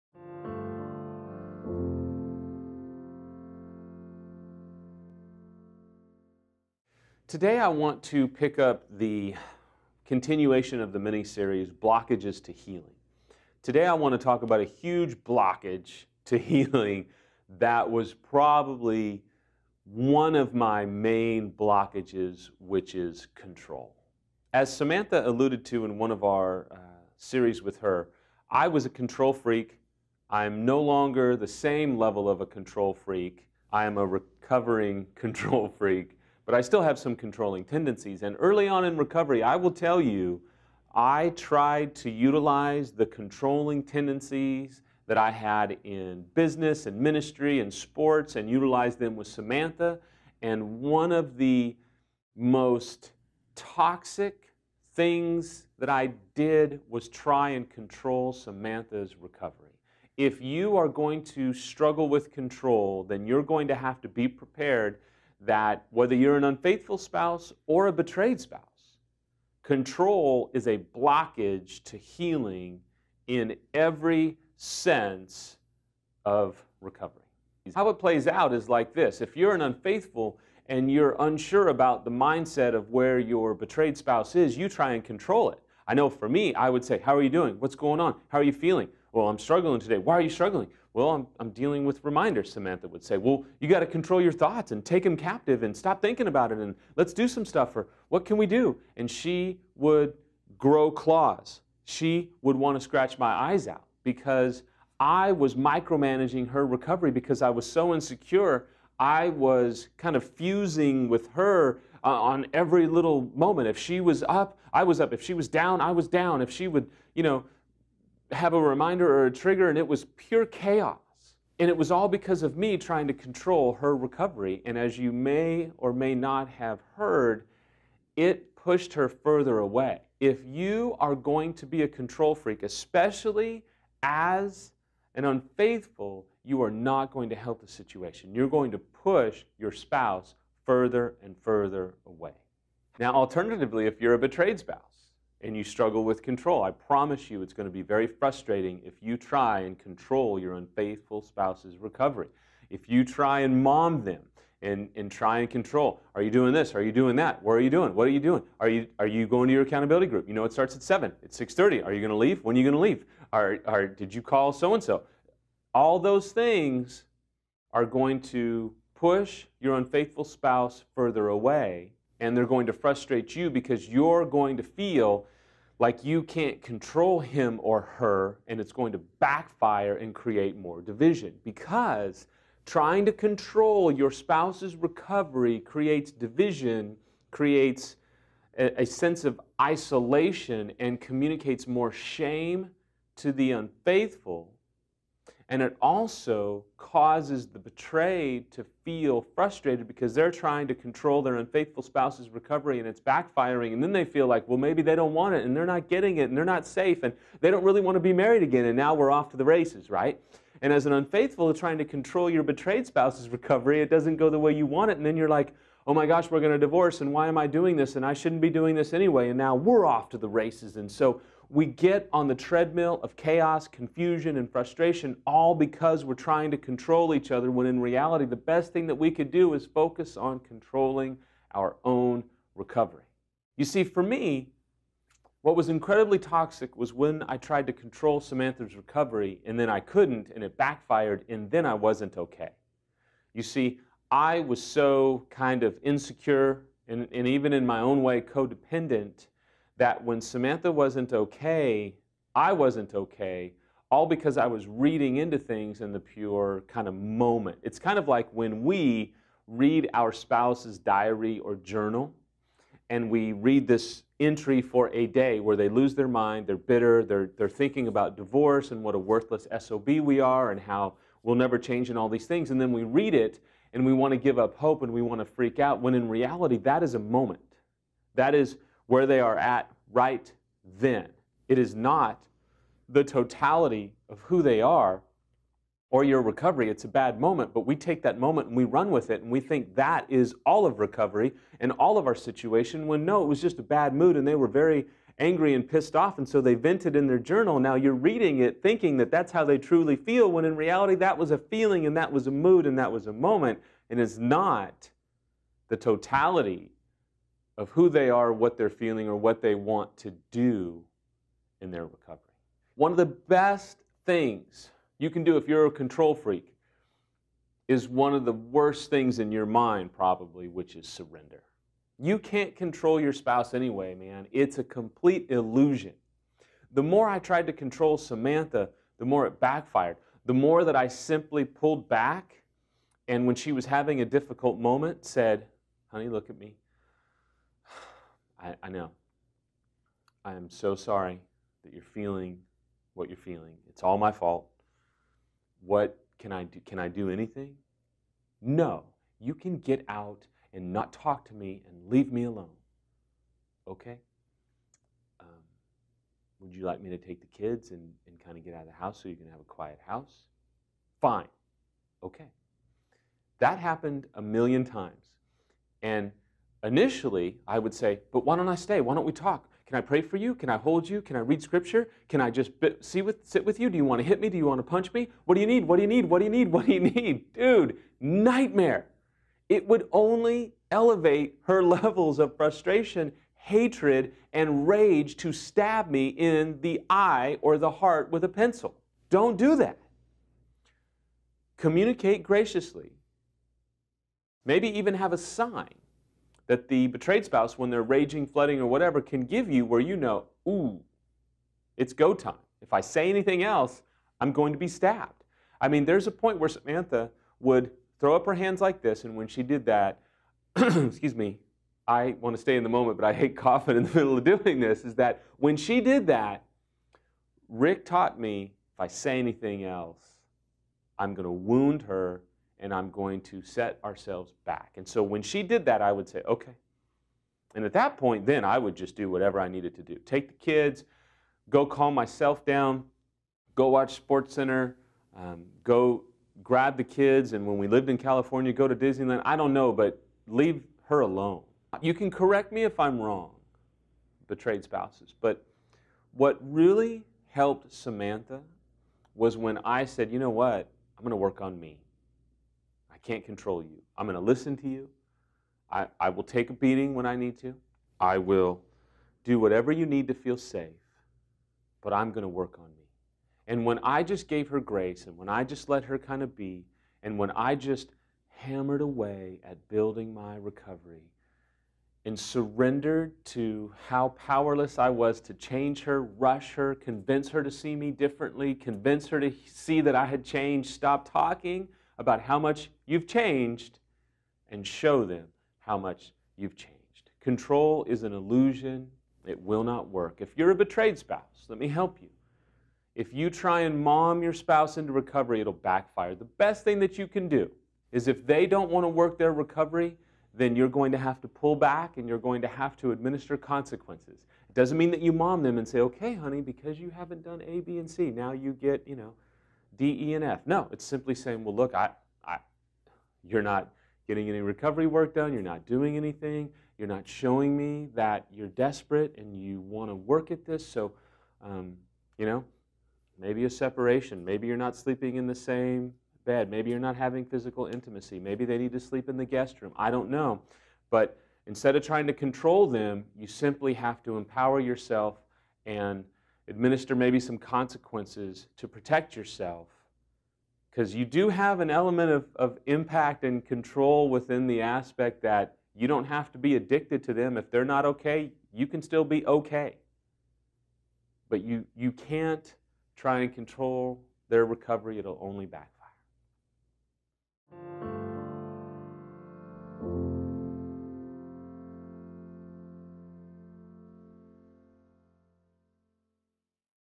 Blockages to Healing Part 2
Join our Recovery Library and gain access to 3,000+ expert resources

Samuel continues his mini-series on what mindsets prevent healing in recovery.